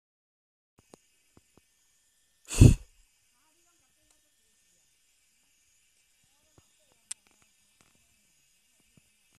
枯葉蛾科（Lasiocampidae） 幼蟲 受驚嚇的反應!! Lebeda nobilis
多紋枯葉蛾? 或 大灰枯葉蛾  ? 台灣櫸(雞油樹) 上移動。